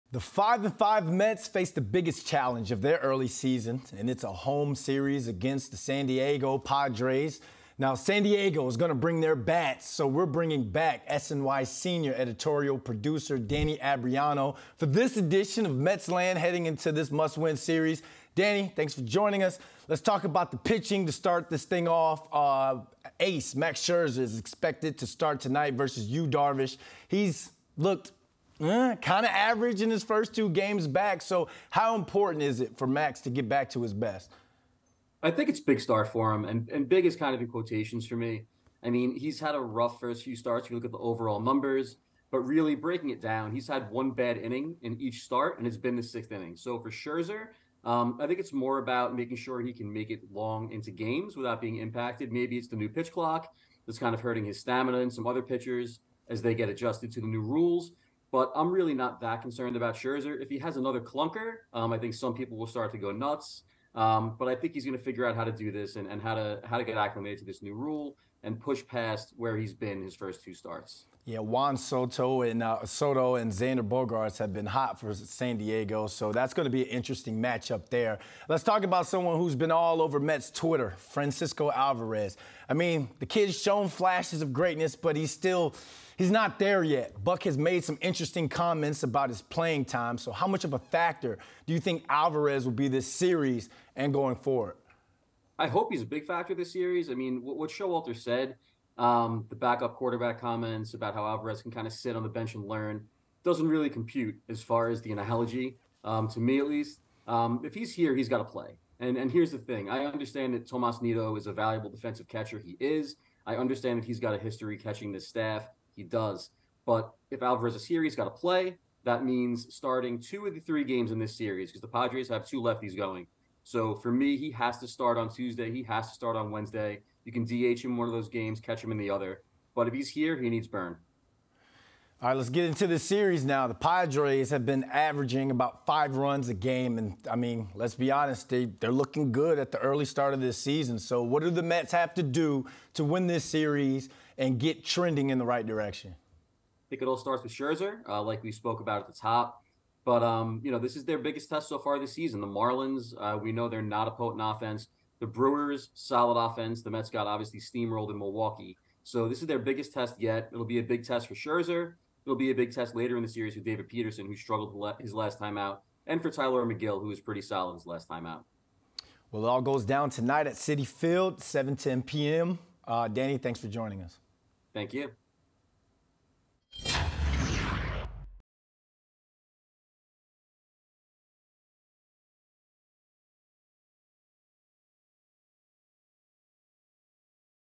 Mets facing Max Scherzer, Francisco Alvarez conundrums ahead of Padres series | New York Post Sports
SNY's Danny Abriano talks Mets’ upcoming series against the Padres, Francisco Alvarez’s potential, Max Scherzer’s struggles with Brandon London.